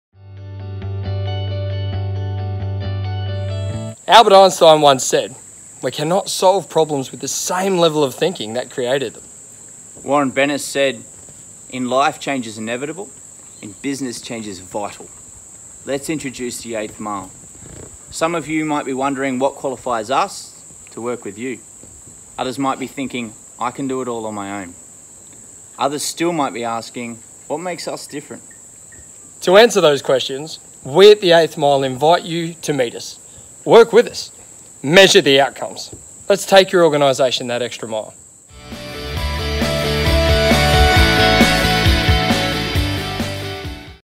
The Eighth Mile Consulting Welcome
David Neal and Jonathan Clark welcome you to The Eighth Mile Consulting. Let us show you why we are different.